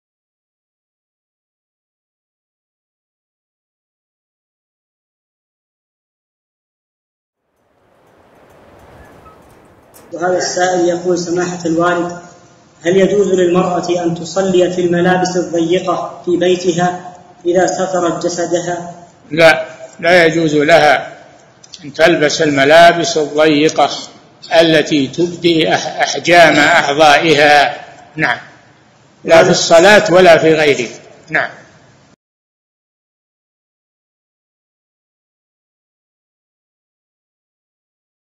Veshja e rrobave të ngushta në namaz dhe jashtë namazit - Feuzan
Imam Salih bin Feuzan el-Feuzan

Pyetje: A i lejohet një gruaje që të falet në shtëpinë e saj me rroba të ngushta nëse ato e mbulojnë trupin e saj?

Përgjigje: Jo, asaj nuk i lejohet që të veshë rroba të ngushta që tregojnë format e trupit të saj, as në namaz e as jashtë namazit.